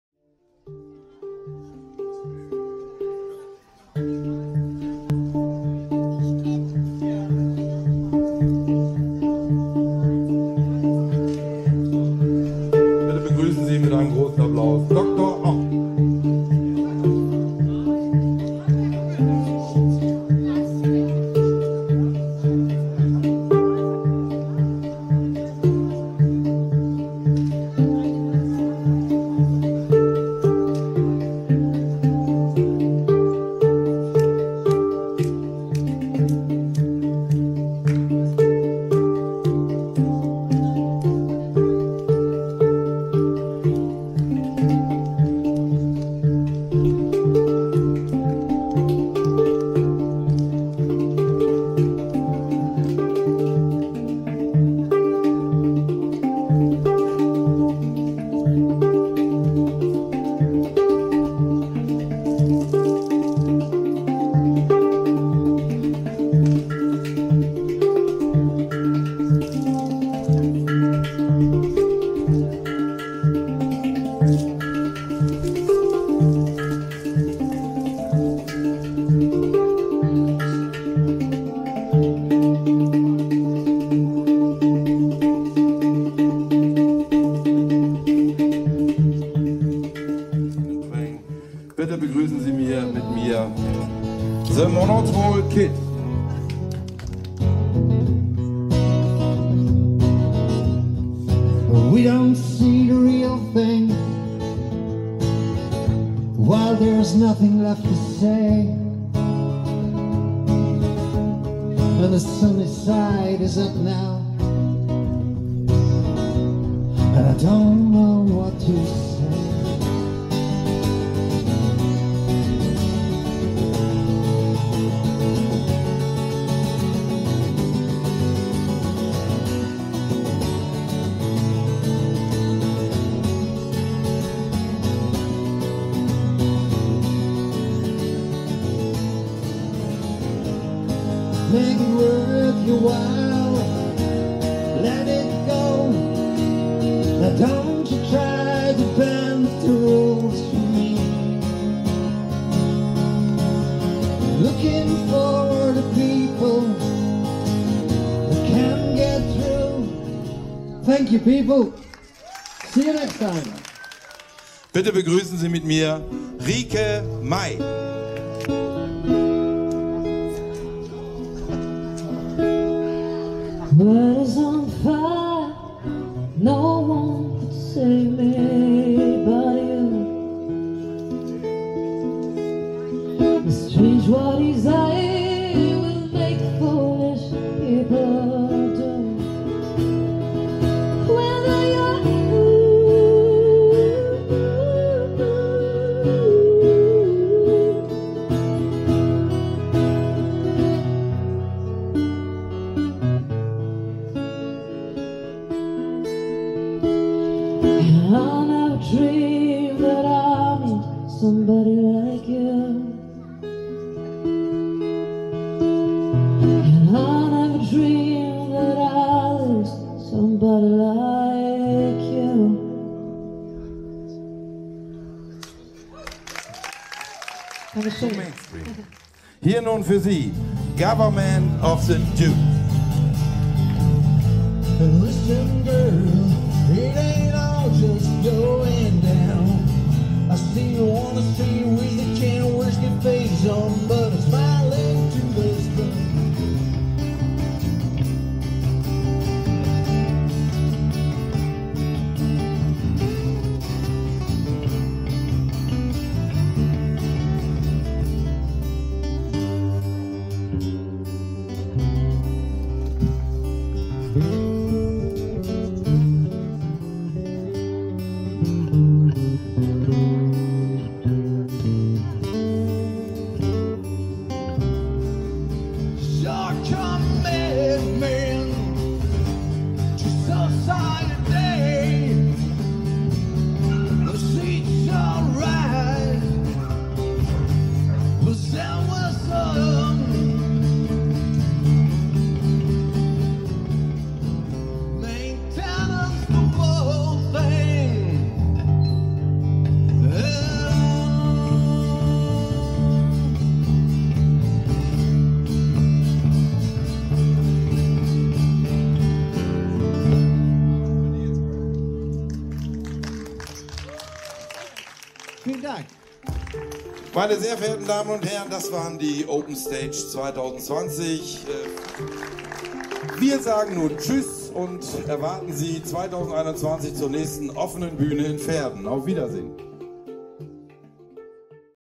Open Stage Verden im Oktober 2020
Durch Corona hat die Stadt Verden im Oktober eine zusätzliche OpenStage genehmigt. Mitwirkende waren The Monotrol Kid, Otto Maier alis Dr Om, Rike Mey und Goverment Of The Dudes. Durch das Programm geführt hat Bernd Maas.Organisiert wurde das Ganze von Jump-i.de, in Person von Jeanette Atherton. Die Technik betreute Glenn Teuber. Danke an alle die sonst so im Einsatz waren !!!